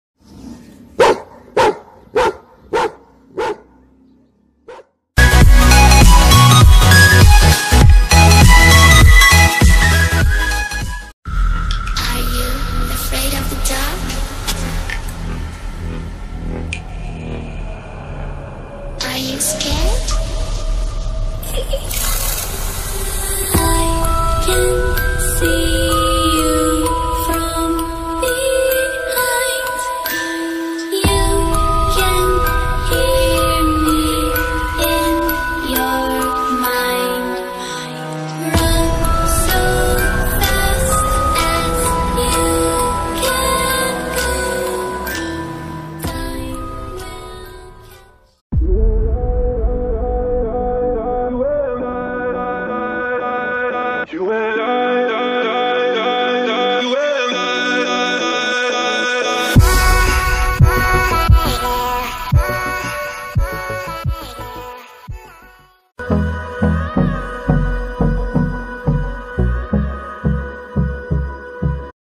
Dangerous saint bernard attack attacking video and playful giant st. Bernard dog
Saint bernard barking video   and also aweome scary creativity with  the effect and also a funny headphone video on the dog